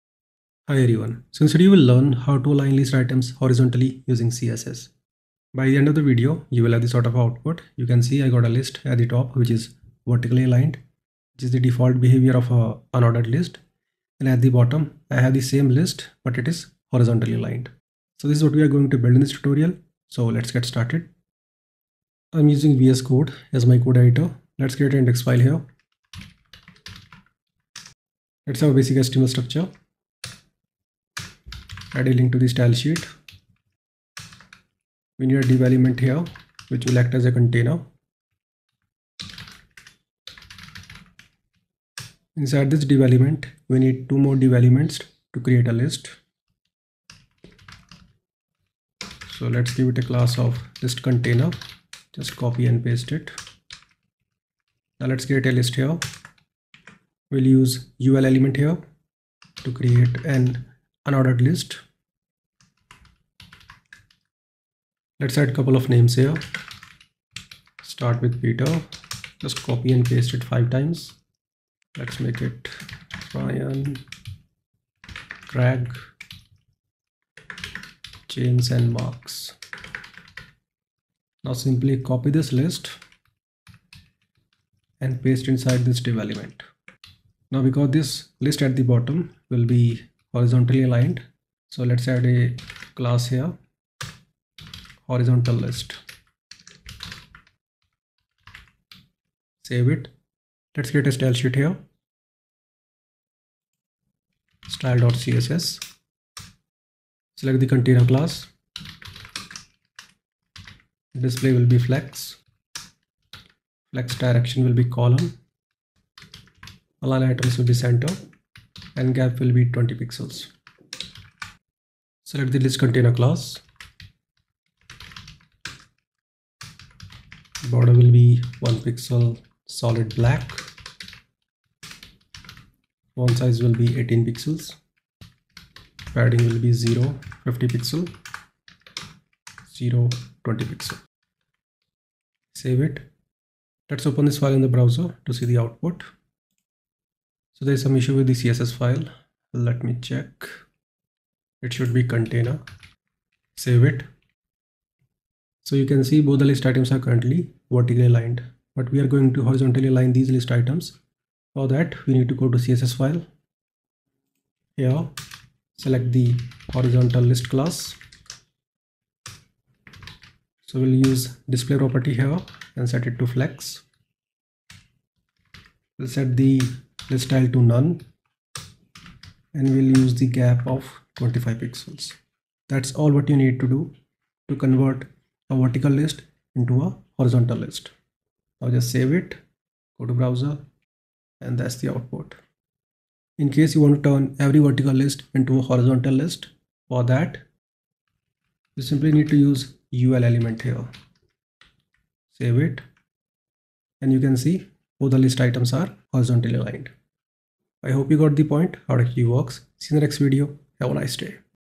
How to Align List Items Horizontally in CSS
In this video, you will learn how to align list items horizontally in css. I hope you'll like the video and leave your feedback and suggestions for future video tutorials.

0:00 Intro
0:21 HTML
1:38 CSS
2:44 Make List Horizontal in CSS
3:12 Final Output